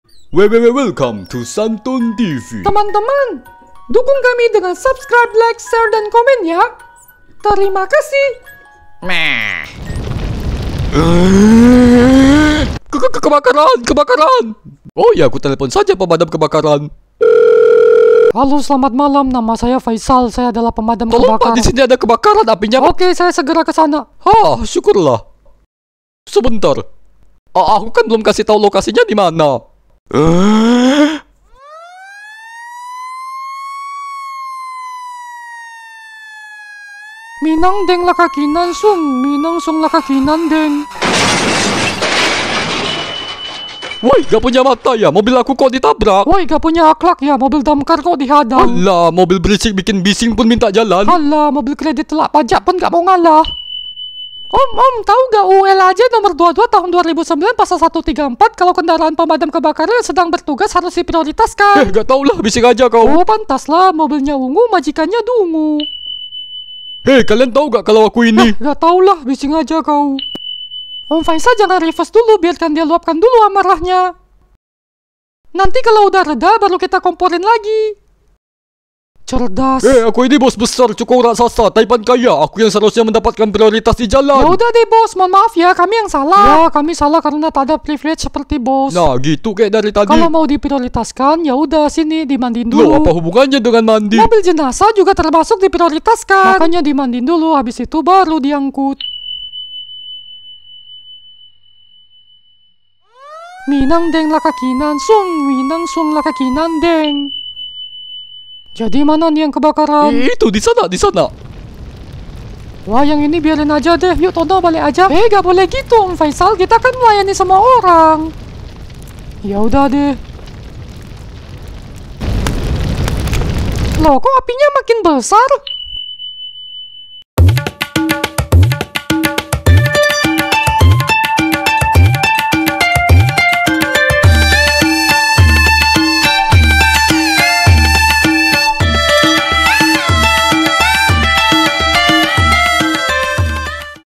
Tono dan Faisal Jadi Pemadam Kebakaran
Episode 191. Kisah Tono dan Faisal yang cosplay menjadi pemadam kebakaran.